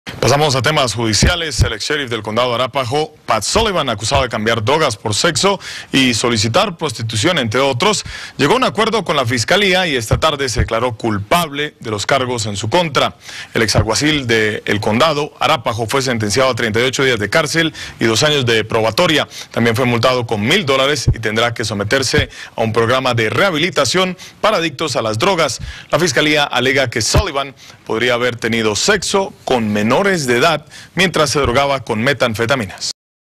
Ex sheriff de Arapahoe sentenciado
El ex agente de la ley en medio del caso de drogas por sexo, es sentenciado luego de un acuerdo con la fiscalía.